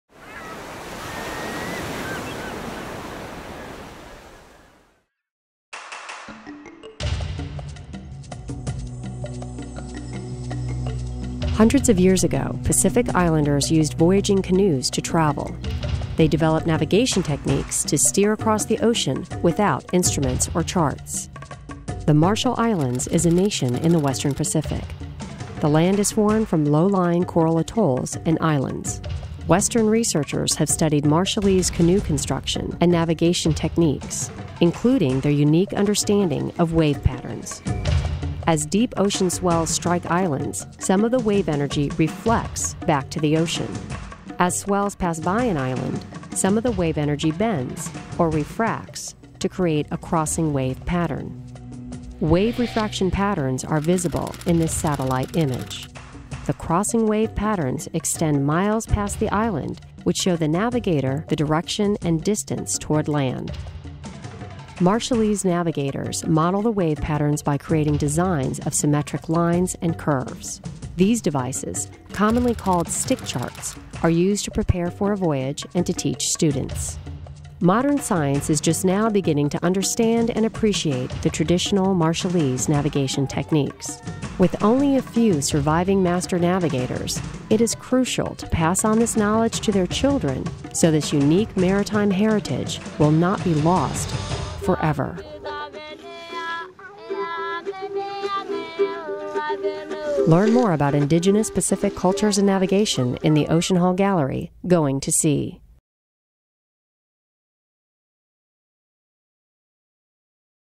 The Ocean Shows Us the Way
Hundreds of years ago, Pacific Islanders used voyaging canoes to travel. They developed navigation techniques to steer across the ocean without instruments or charts. The Marshall Islands is a nation in the western Pacific. The land is formed from low-lying coral atolls and islands.

Western researchers have studied Marshallese canoe construction and navigation techniques, including their unique understanding of wave patterns.

As deep ocean swells strike islands, some of the wave energy reflects back to the ocean. As swells pass by an island, some of the wave energy bends, or refracts, to create a crossing wave pattern.

Wave refraction patterns are visible in this satellite image. The crossing wave patterns extend miles past the island, which show the navigator the direction and distance toward land.

Marshallese navigators model the wave patterns by creating designs of symmetric lines and curves. These devices, commonly called "stick charts," are used to prepare for a voyage and to teach students.

Modern science is just now beginning to understand and appreciate the traditional Marshallese navigation techniques. With only a few surviving master navigators, it is crucial to pass on this knowledge to their children so this unique maritime heritage will not be lost forever.

Credits:
NOAA Ocean and Atmospheric Research.
University of Hawaii Sea Grant College Program.
Wenner-Gren Foundation for Anthropological Research.
National Science Foundation.
Waan Aelon in Majel.